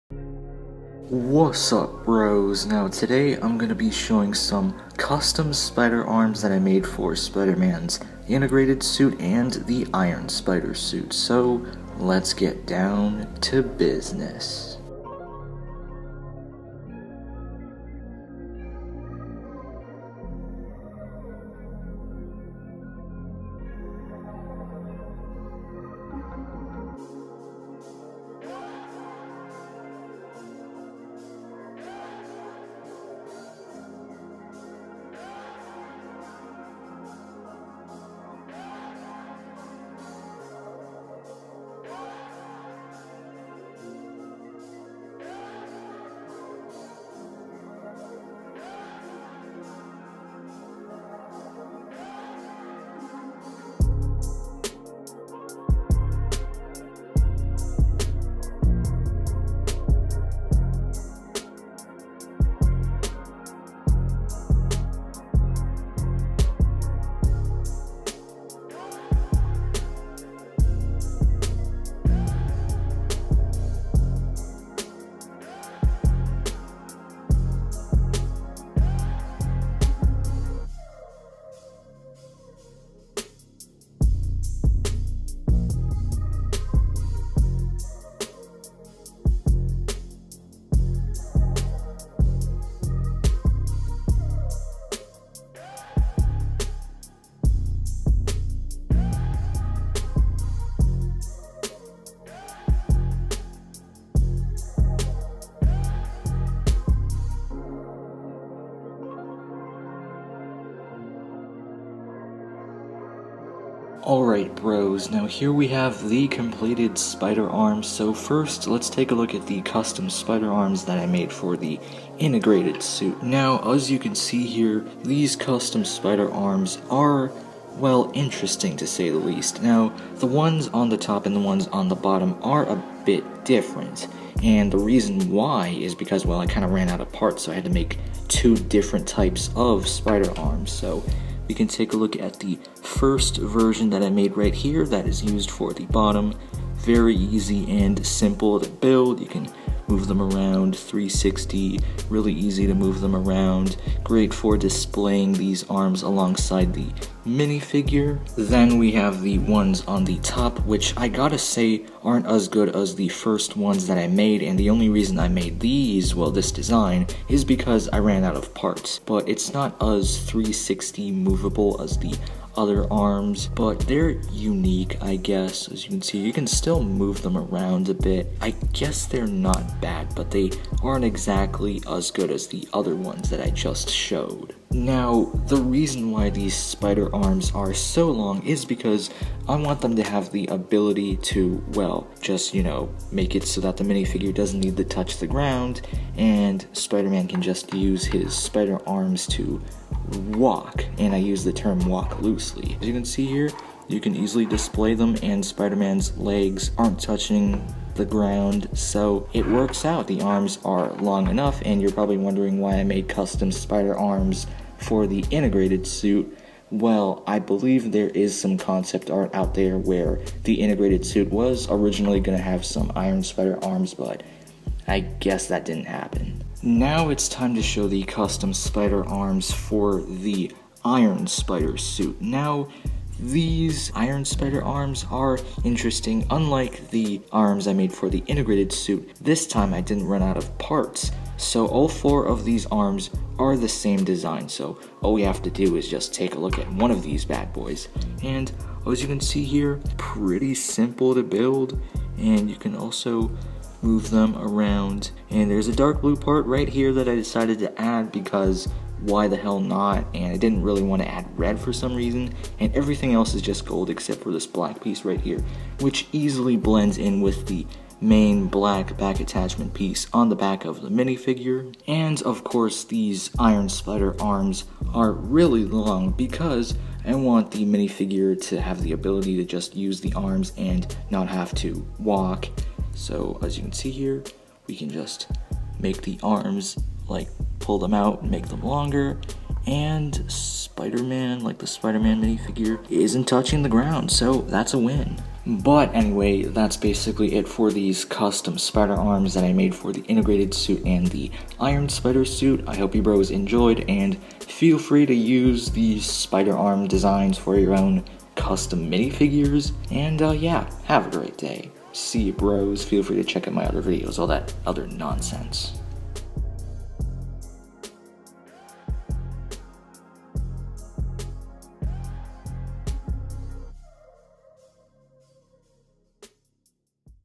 Lego Spider-Man Integrated Suit & Iron Spider Upgraded Spider Arms
0:00 Introduction
0:15 Speed Build
1:43 Cinematic Shots
1:54 Explanation/Features
4:55 Outro Music used by Karl Casey/White Bat Audio